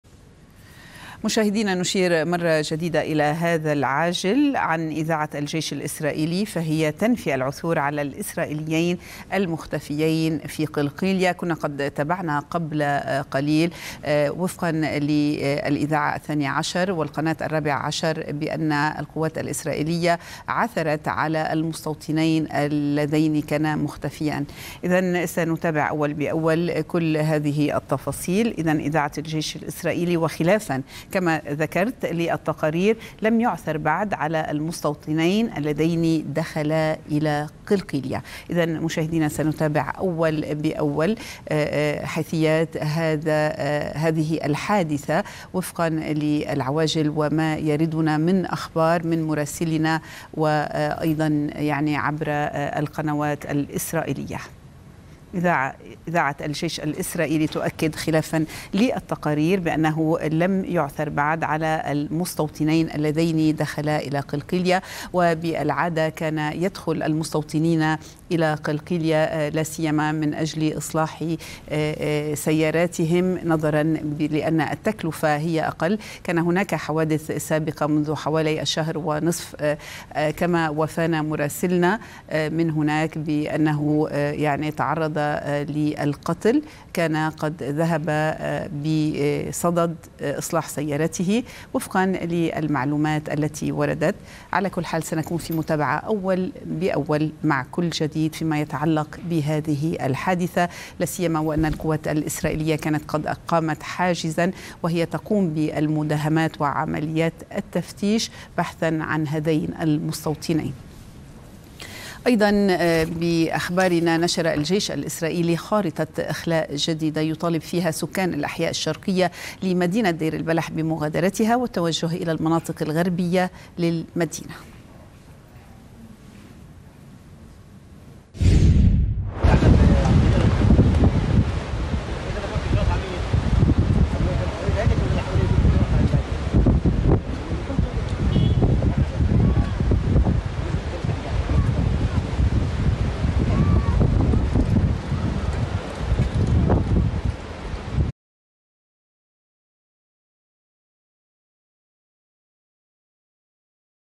إسرائيل تنفي العثور على المستوطنين اللذين دخلا قلقيلية
إذاعة الجيش الإسرائيلي تنفي العثور على المستوطنين اللذين دخلا قلقيلية في الضفة_الغربية حتى الآن